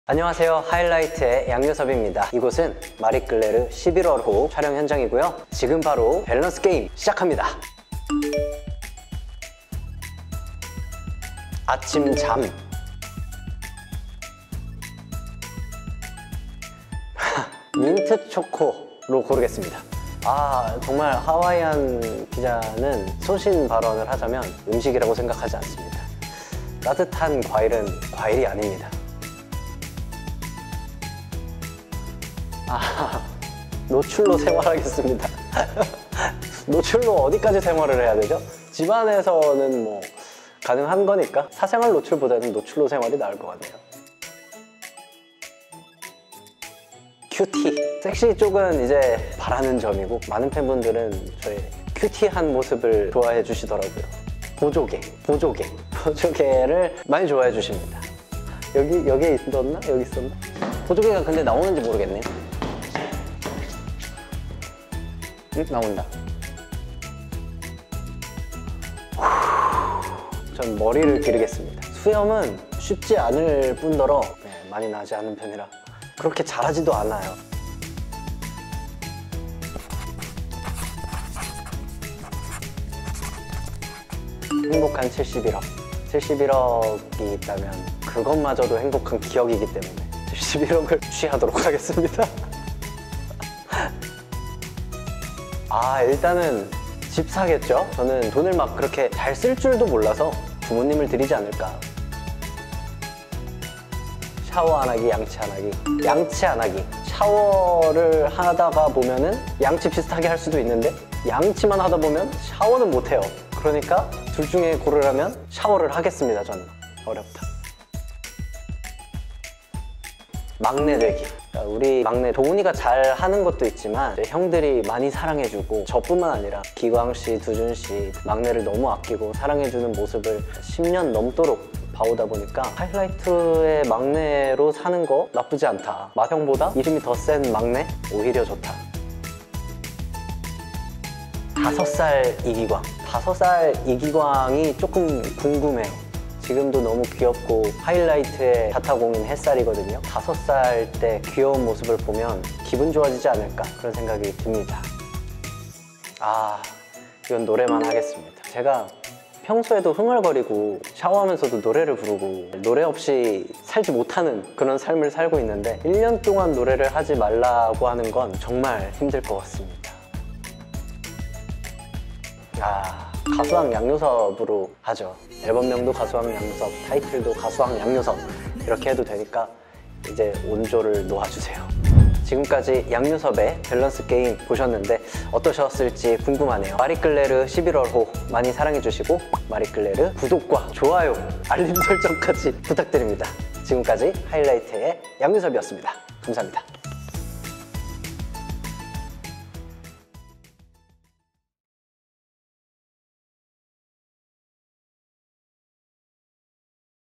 저 양요섭 오늘 소신 발언하고 가겠습니다🍍🍕 Balance game with Yang Yo Seop(Eng Sub)
가수왕 양요섭과 함께한 마리끌레르 밸런스게임😉
5살 이기광 vs 이기광 5명
하이라이트 막내 되기 vs 맏형 되기
쪼꼬미즈 케미부터 막내 우쭈쭈 모멘트까지
하이라이트 애정으로 가득 담긴 그의 밸런스 게임을 지금 바로 영상으로 만나보세요🙌